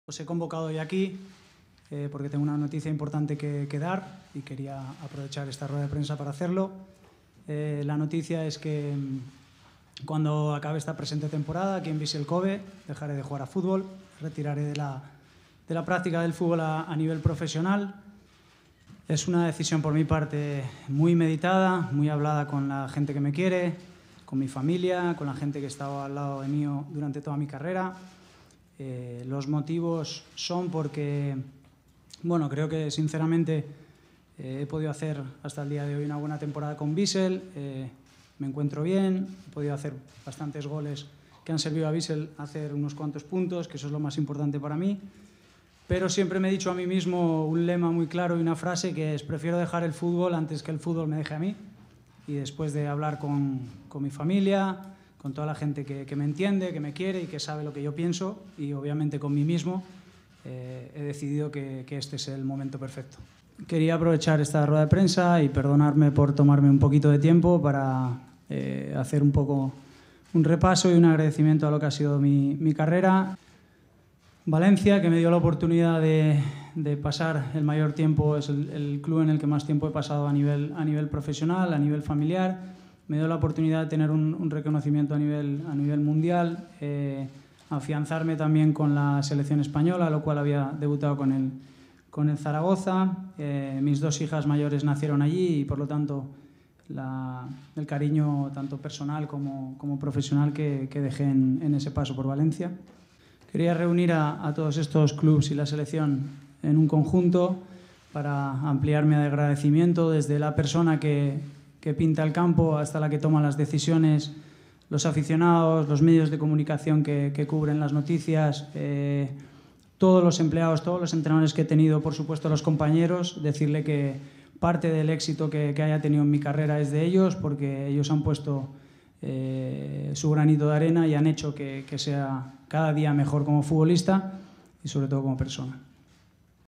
DAVID VILLA: “EL VALENCIA CF ME DIO LA OPORTUNIDAD DE TENER UN RECONOCIMIENTO A NIVEL MUNDIAL”
El asturiano ha anunciado que a final de curso con el Vissel Kobe deja el fútbol y reconoce que su paso por el Valencia CF fue clave en su carrera.